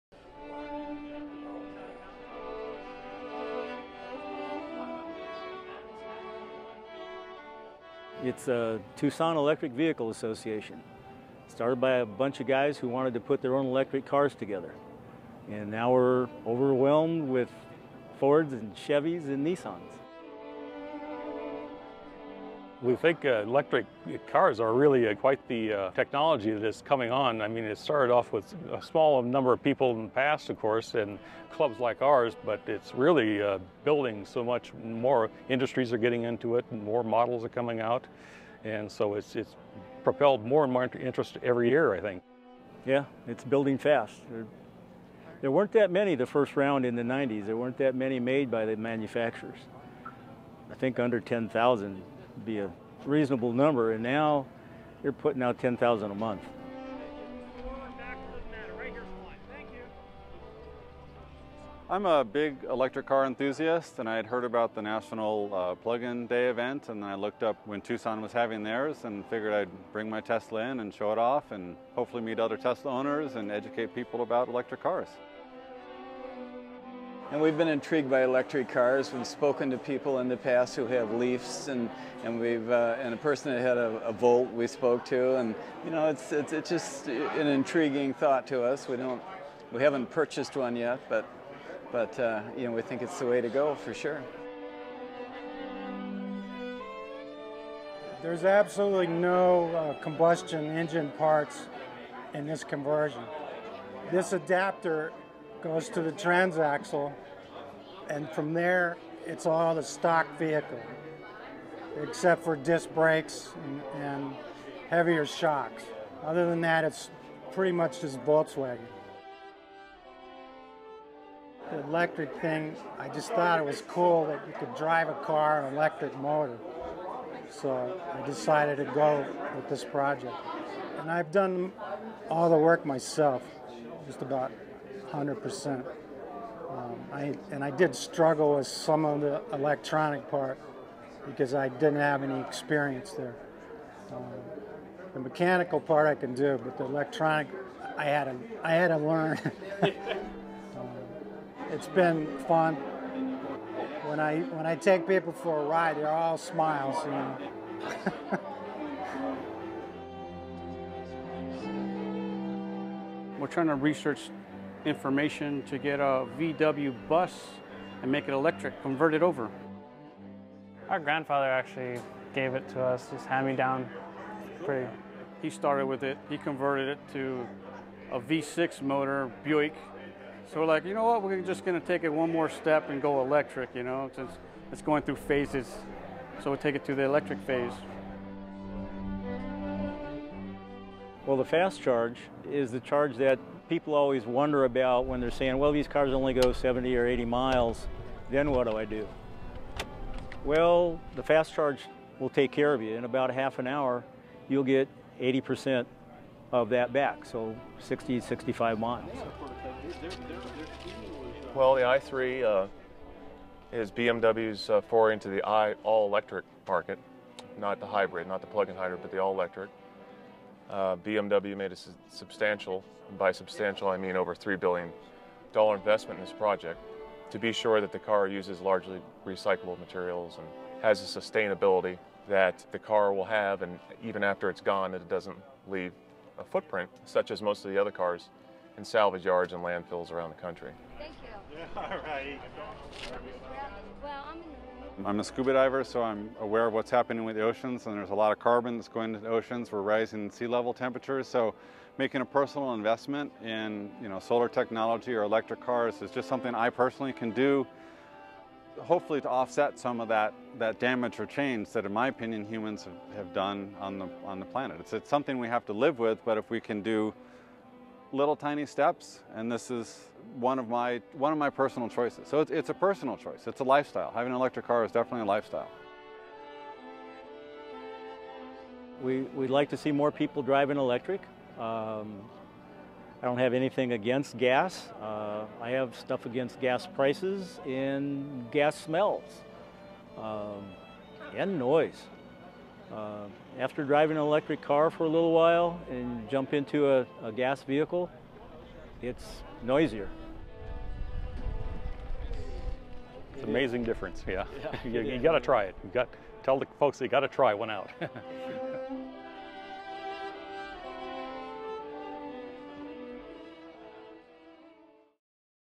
Tucson Plugs In 2013
The camera crew from KUAT/PBS6 (Arizona Public Media) came to our National Plug In Day (NPID) event in Tucson on October 29, 2013. I've been involved with NPID since it's inception in the fall of 2011 and have called each of these gathering Tucson Plugs In.

Local organization comes from TEVA2, the Tucson Electric Vehicle Association and, nationally, from the Electric Auto Association, Plug In America and the Sierra Club

The Tucson Plugs In 2013 occurred at the Bookman's Sports Exchange, 3330 E. Speedway Blvd. Highlights included the inauguration of GoE3 quick charging terminal and the Arizona debut of the BMW i3 electric car.